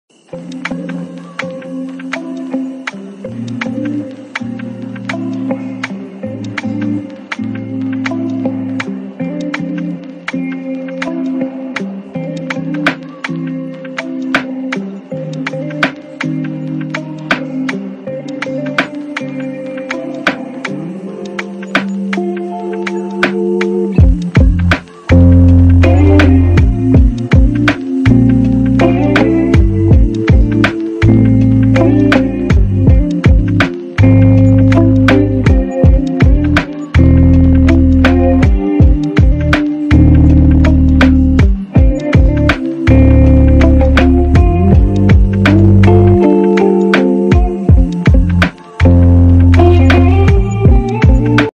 unboxing my new buddha statue
Take a look at this AELANA GRAPHY Polyresin Gautam Buddha Statue for Home Decor Big Size Decorative Showpiece  -  24 cm on Flipkart